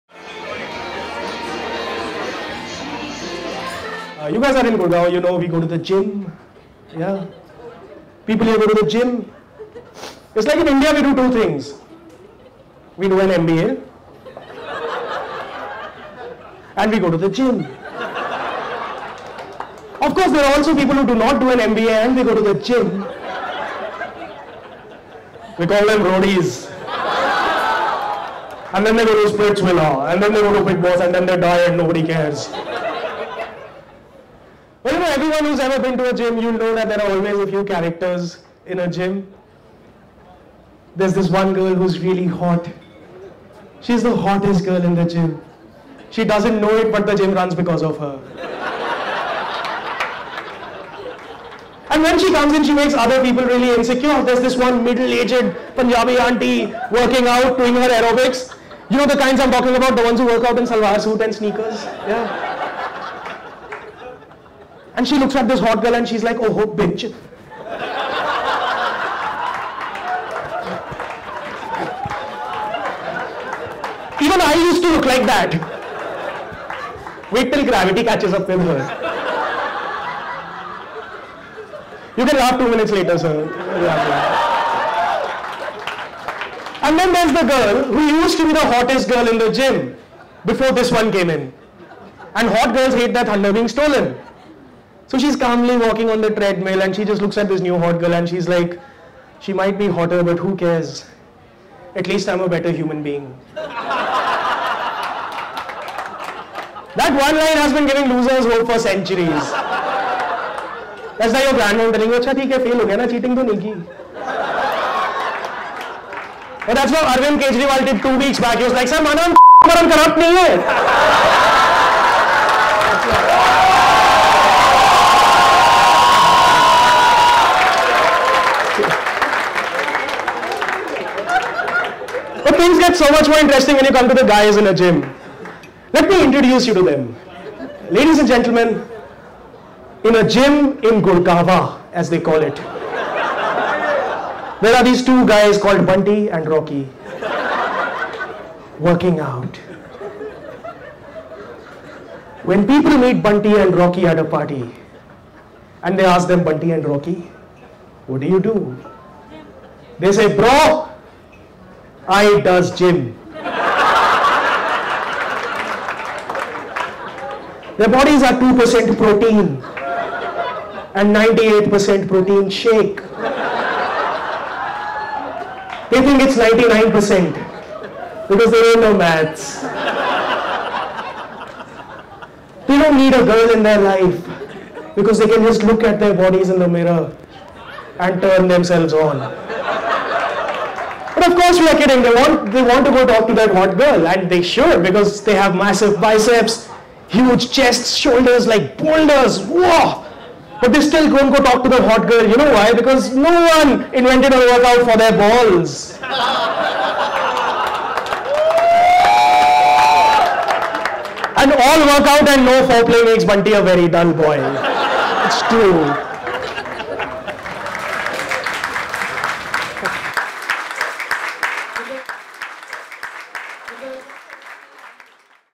The Truth About Gyms-Stand Up Comedy| Vikramjit Singh
Indian Stand Up Comedian Vikramjit Singh explores the truth about gyms, and the most interesting people found in every gym.